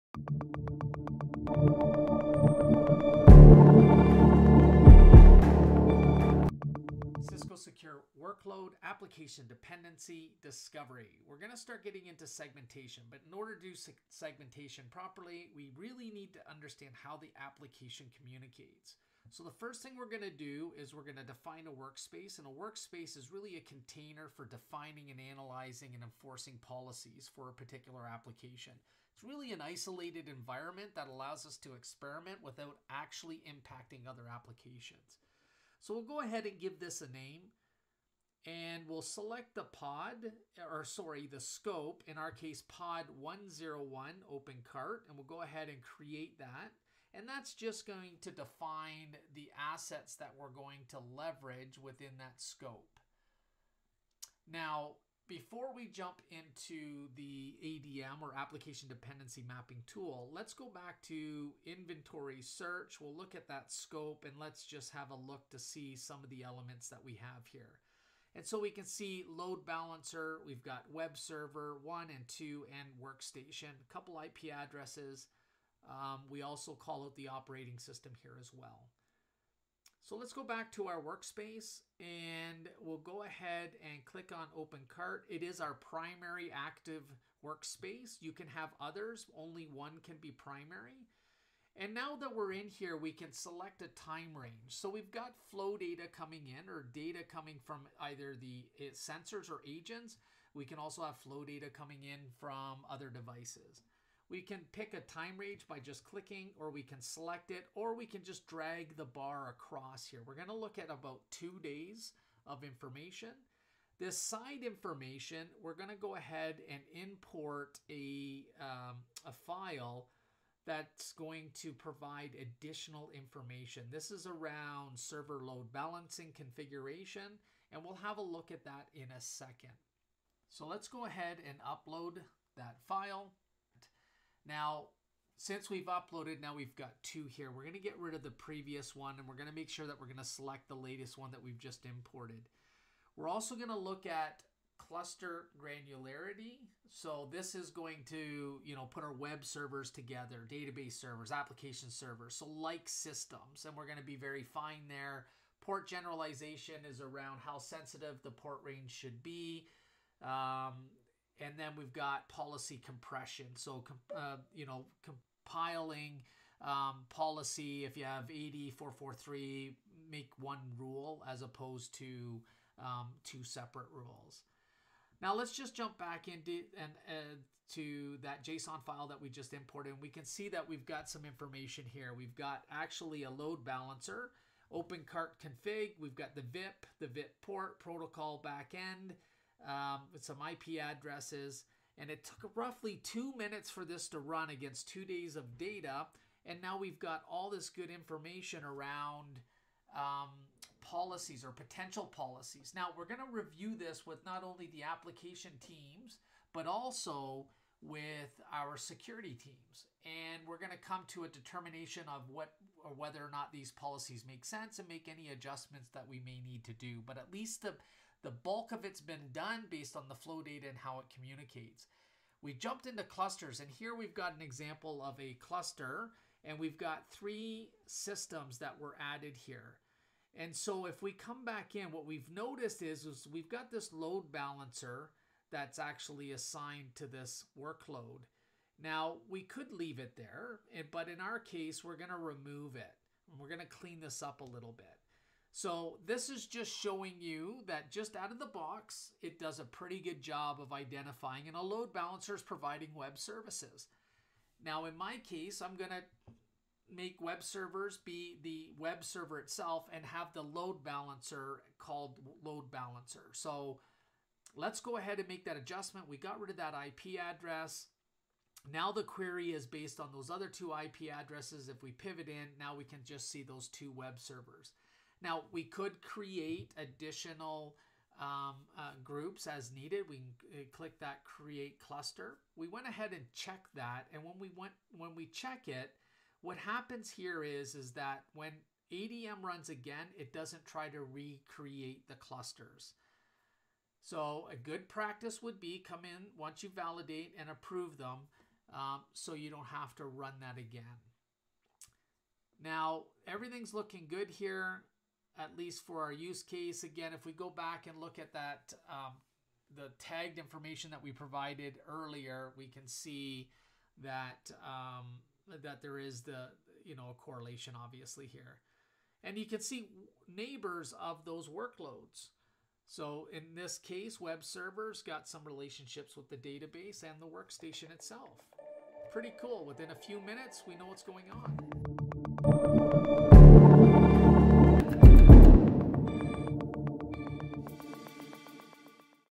Cisco Secure Workload: Application Dependency Mapping Discovery
In this video we will leverage application dependency mapping (ADM) to identify what systems communicate on what ports. This is a major step towards segmentation as it is very difficult to do unless you understand that applications. ADM builds policies for your review streamlining the process and once confirm you can move into enforcement or what you call micro/application segmentation.